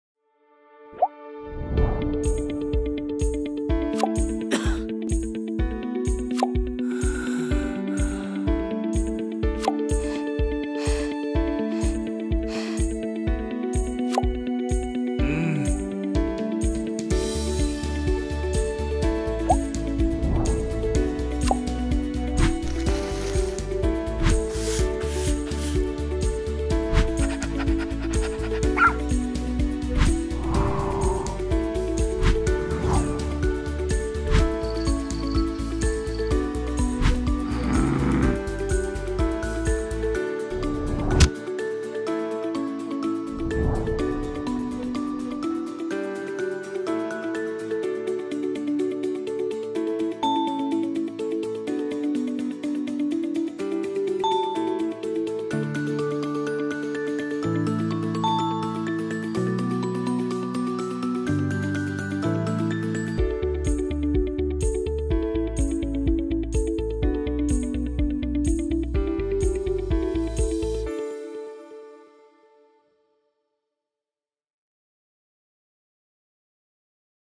Is it Asthma? | American Academy of Pediatrics (AAP)
In this animated video from the American Academy of Pediatrics, learn more about the symptoms of asthma, what triggers can cause flare ups and why an Asthma Action Plan is so important.